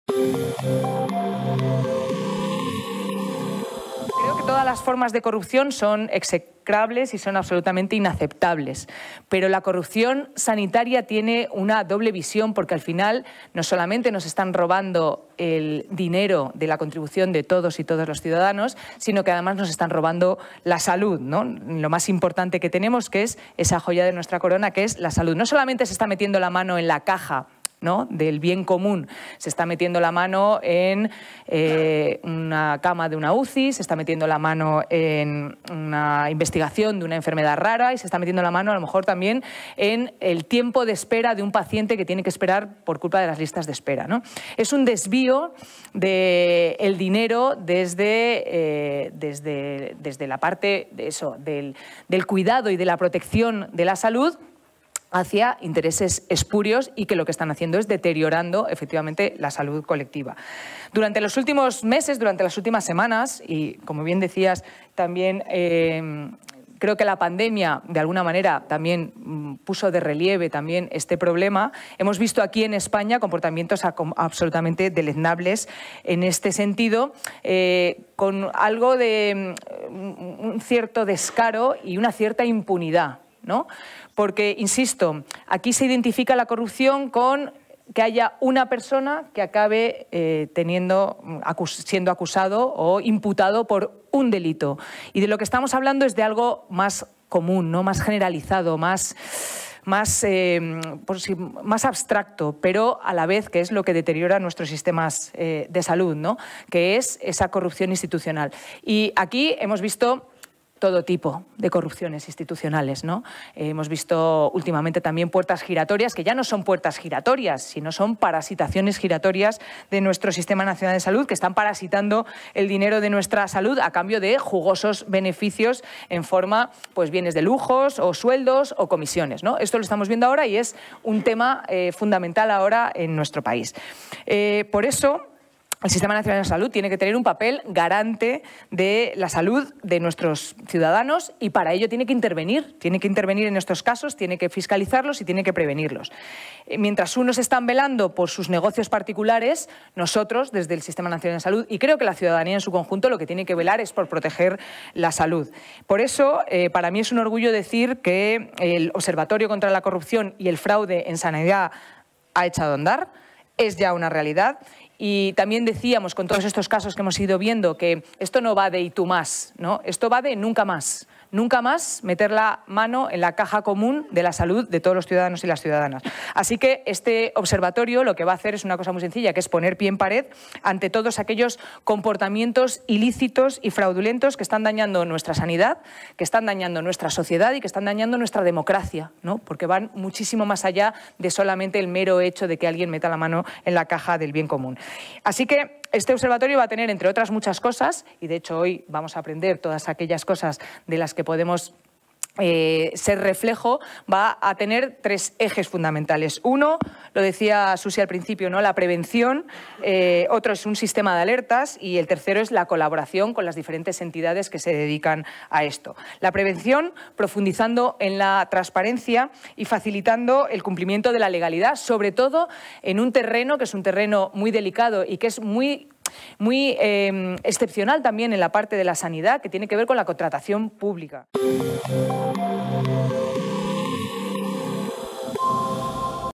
García: "El observatorio contra la corrupción sanitaria ya es una realidad y está operando"
La ministra de Sanidad ha presentado la actividad de este nuevo observatorio contra el fraude que trabajará por la prevención y la alerta de casos